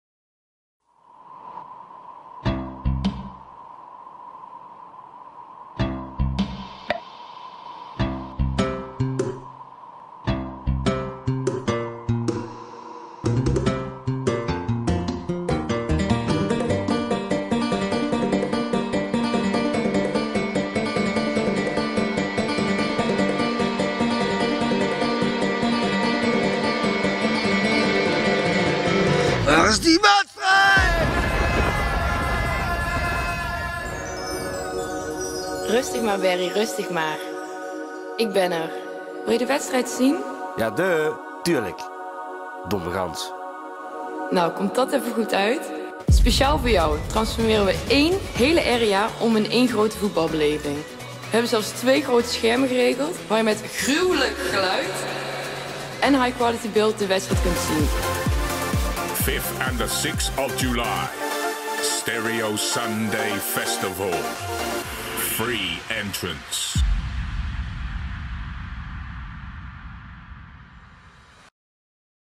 Stereo Sunday 2014 - OP GROOT SCHERM WK KIJKEN
Stereo Sunday, GRATIS dancefestival op 5 + 6 juli, heeft naast drie areas vol house, eclectic, techno en hardstyle ook een speciaal WK momentje. Op zaterdag 5 juli kun je met al je vrienden op GROOT scherm en met GOED geluid de WK wedstrijd van ORANJE komen kijken. Zonnetje, biertje, snackje, lekkere muziek en LIVE met al je vrienden voetbal kijken in het park... Wat wil je nog meer?!